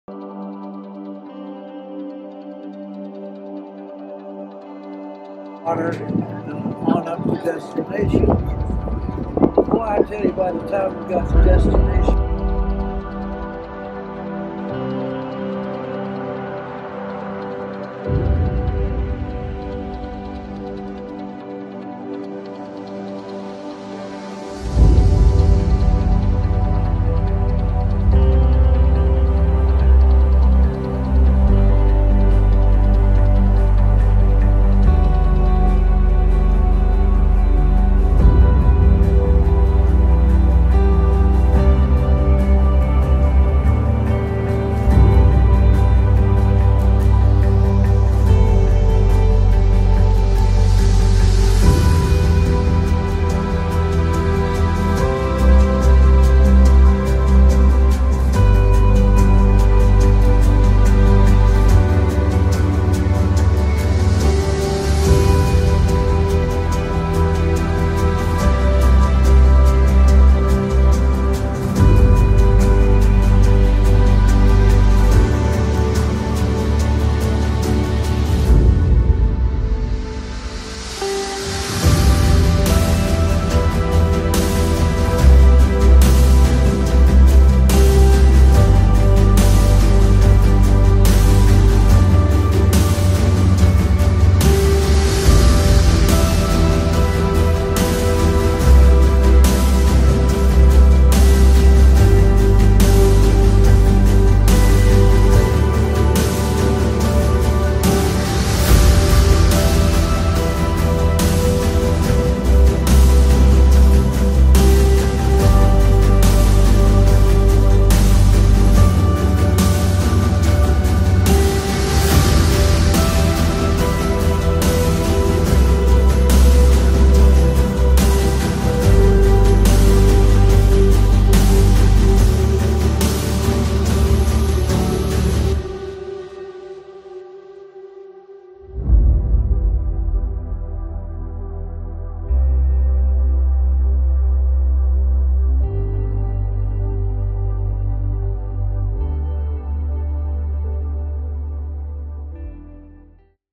Veterans Day 2017
Tribute Video to my fellow Veterans.  WW2 Bomber Planes, P 51 Mustang, Local Veteran Memorials, Mountain View, CA; Cupertino, CA; Washington, D.C.